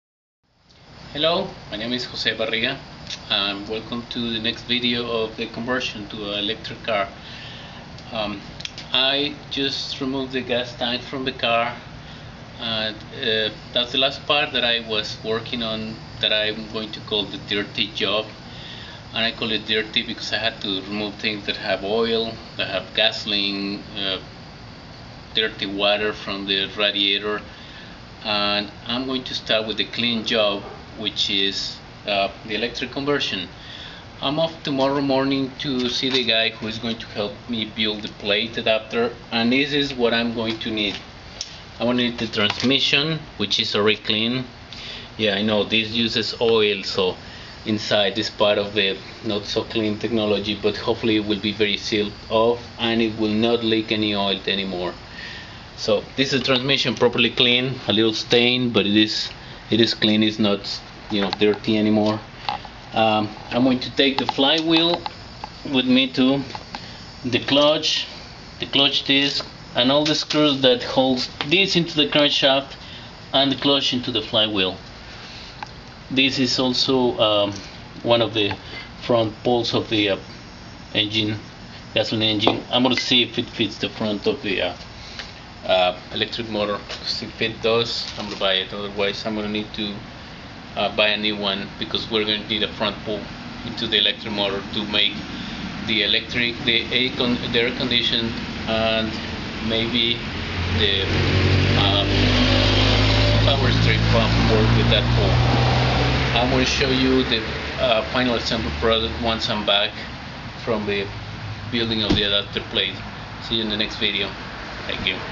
Nissan Sentra Electric Car Conversion Part 9
A video series of converting a Nissan Sentra car into an electric car.